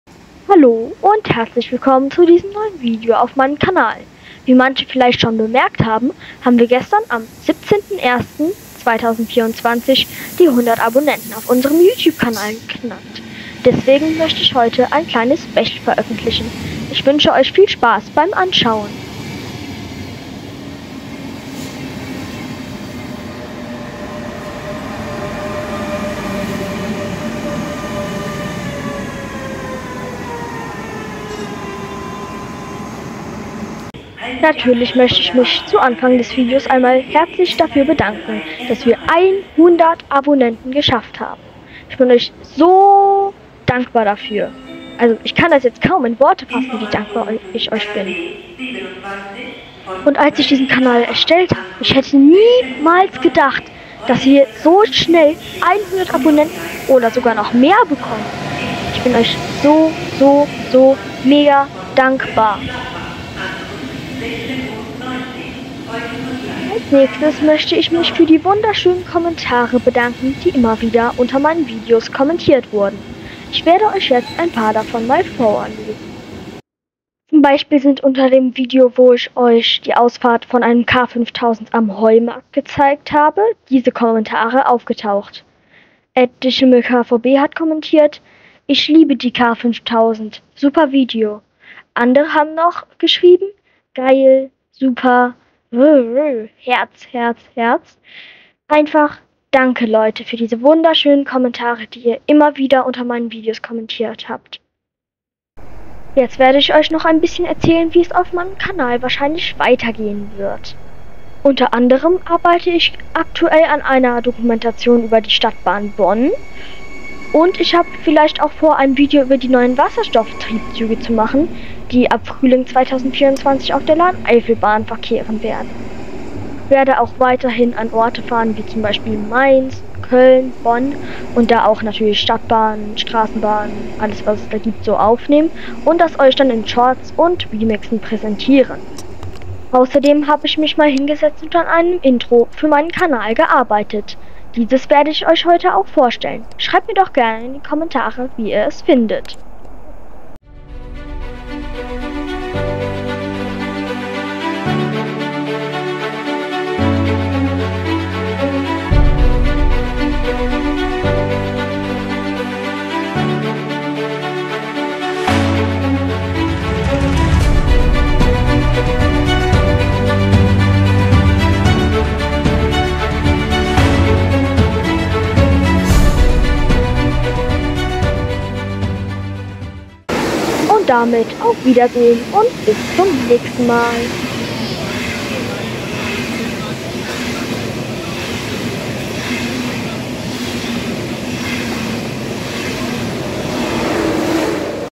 Danke für 100 Abonnenten :D - Mein Special
Heute präsentiere ich euch mein 100 Abo Special, in dem ihr mein neues Kanalintro und viel mehr sehen werdet!
Viel Spaß beim Anschauen!!! Und nochmal Danke!